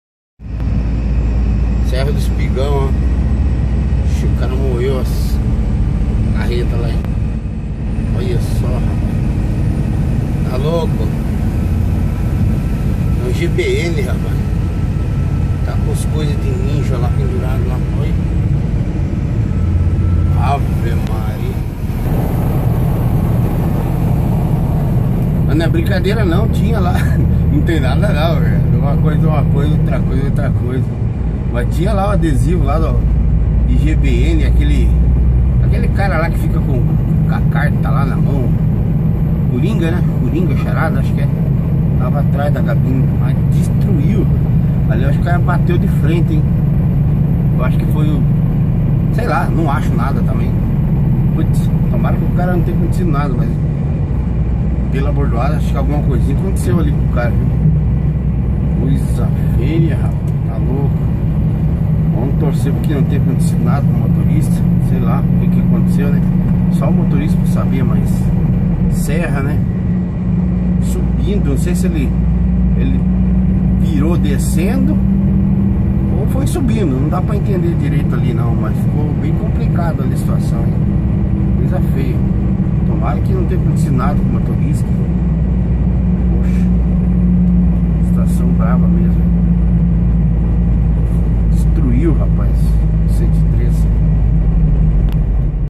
Perda total na Scania 113. Carreta desceu e derrub
Lamentável. (GBN é uma brincadeira ok) Diário de bordo de um caminhoneiro